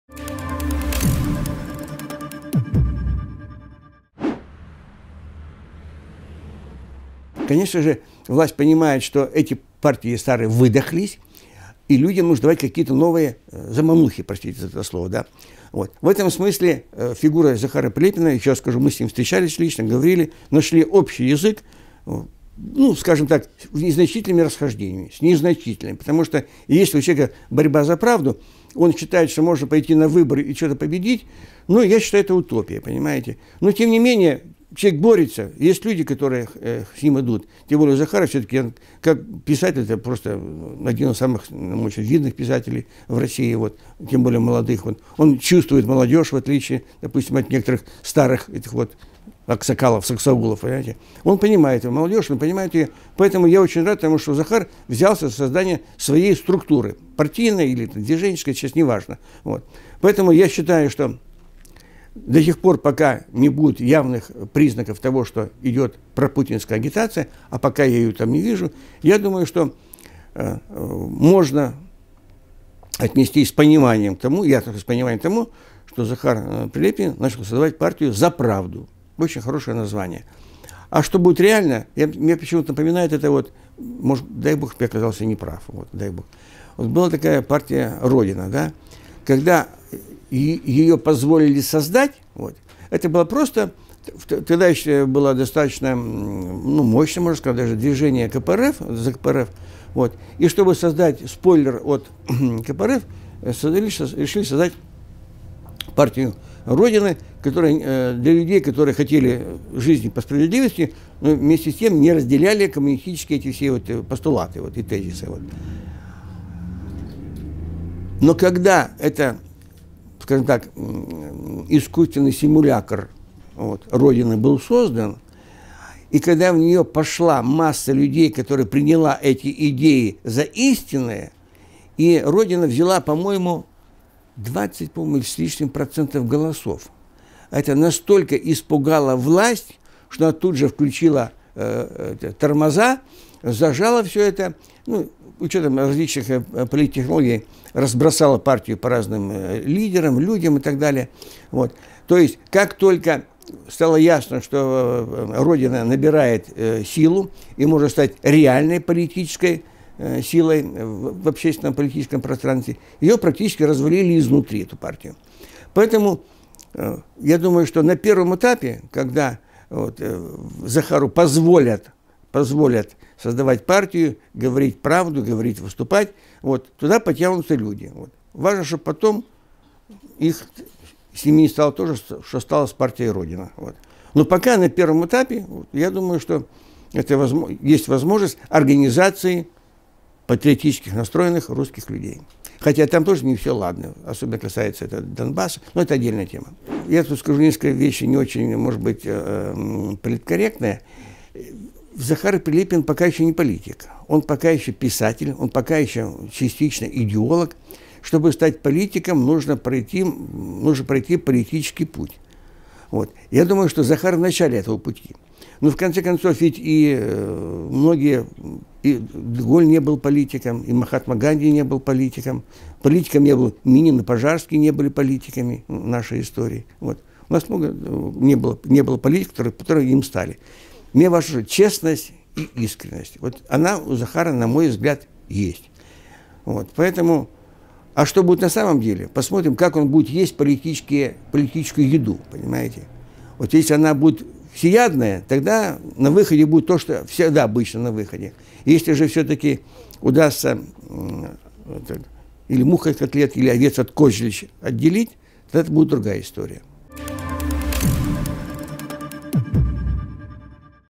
Владимир Квачков прямолинейно о Захаре Прилепине
Полковник ГРУ в отставке Владимир Квачков ответил на вопрос как он относится к Захару Прилепину и его партии «За Правду».